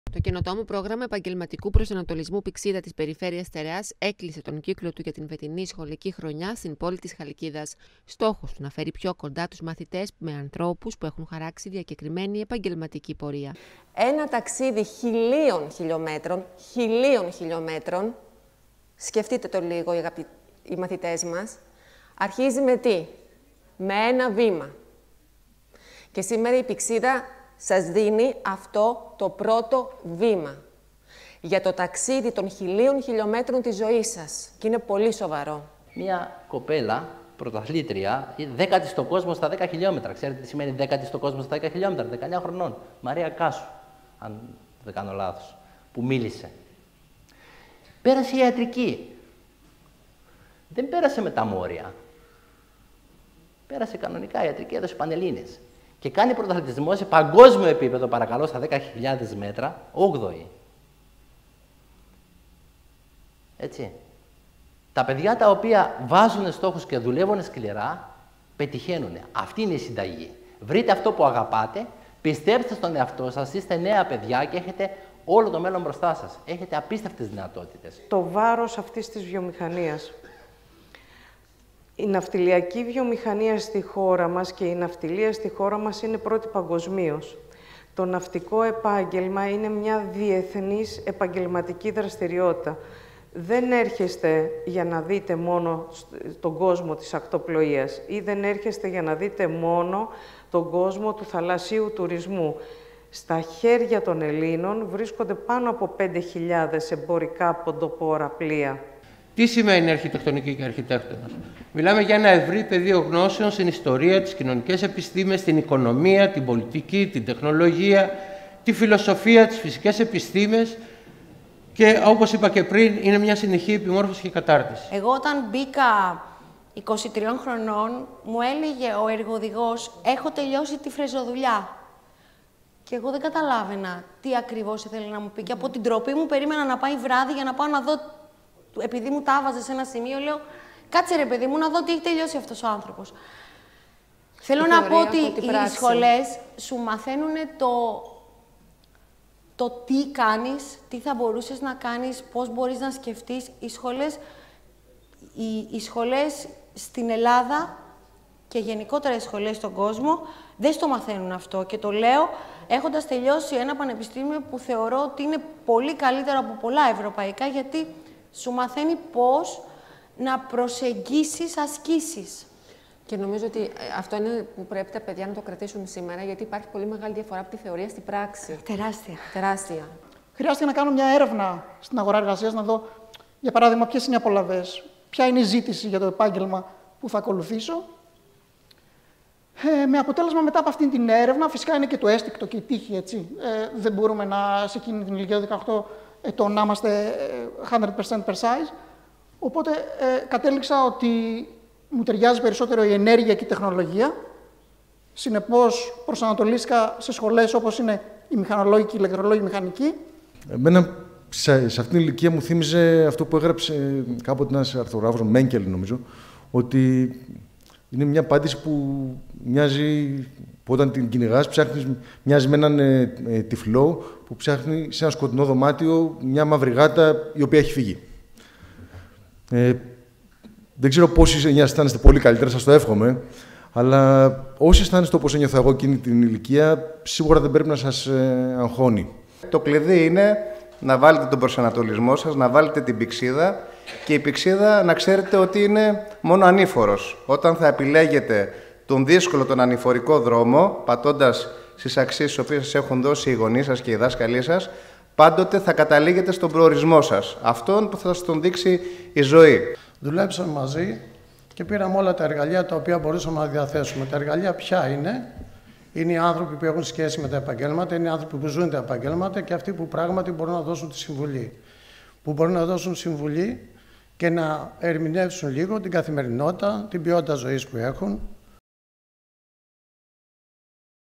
Ημερίδα για τον επαγγελματικό προσανατολισμό
Στη Χαλκίδα έκλεισε ο κύκλος του προγράμματος ΠΥΞΙΔΑ της Περιφέρειας Στερεάς για την φετινή σχολική περίοδο. Η ημερίδα αποτελούνταν από έξι ενότητες κατά τις οποίες ακαδημαϊκοί και εξειδικευμένοι επαγγελματίες ανέπτυσσαν επαγγέλματα που αφορούσαν στην ενέργεια, τη βιομηχανία, την αρχιτεκτονικά και τα ναυτιλιακά.